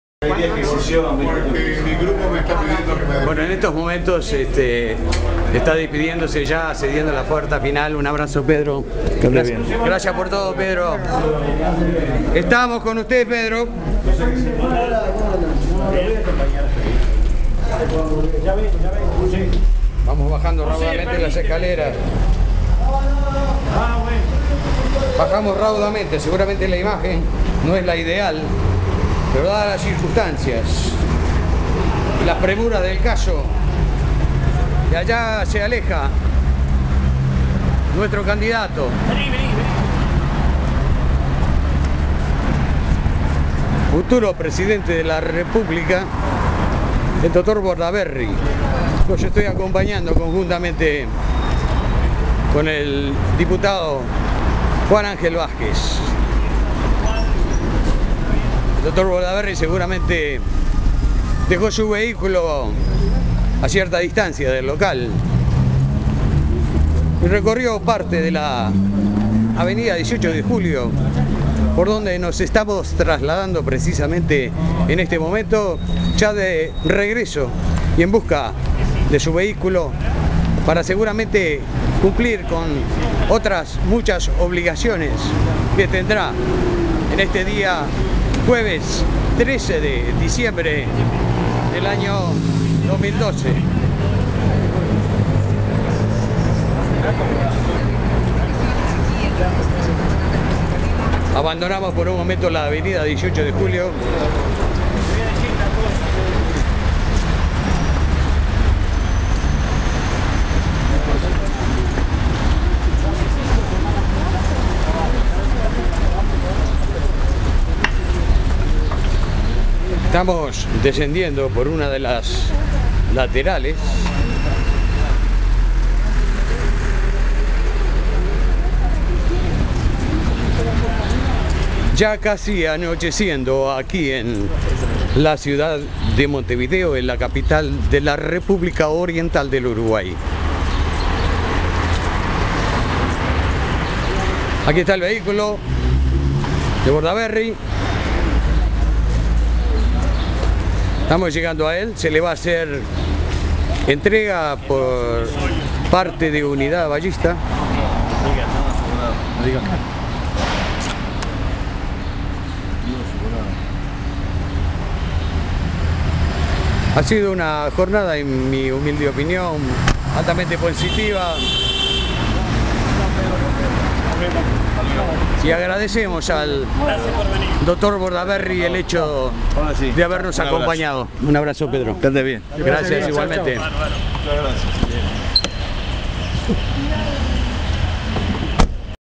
Juan Angel Vázquez despide el 2012 con Pedro Bordaberry - 22
En la Sede Central de Unidad Batllista del Partido Colorado, sita en la avenida 18 de julio 2109, agrupación liderada por el Diputado Juan Angel Vázquez, se celebró un muy nutrido acto que desbordó la capacidad locativa de la misma, y en el cual tuvimos la grata visita del líder de Vamos Uruguay, Dr. Pedro Bordaberry. A posterior del espacio destinado al uso de la palabra, lo hizo en primera instancia el dueño de casa, luego la señorita Patricia Quiroga en nombre de la juventud y el Dr. Bordaberry, compartimos un brindis deseándonos todos muchas felicidades y el deseo de un mejor año de actividad política que nos facilite entre todos ir incrementando las filas de un movimiento que permita al Partido recuperar el poder en las elecciones del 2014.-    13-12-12